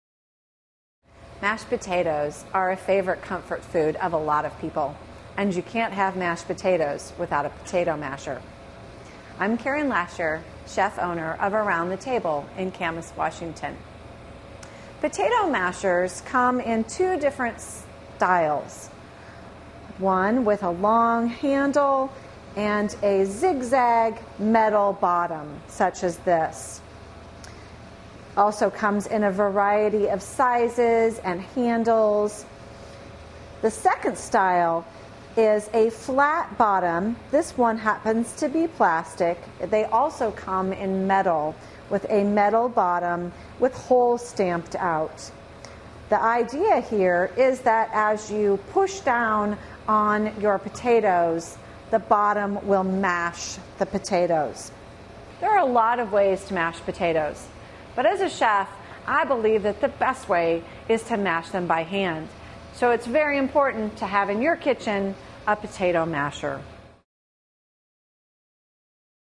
History of the Wire Potato Masher
History of the Wire Potato Masher. Part of the series: Food History. Wire potato mashers are available in a variety of materials, shapes and sizes, including the long-handled zigzag wire masher and the flat-bottomed masher with holes. Find the right potato masher to make a great comfort dish with information from a professional catering chef in this free video on food.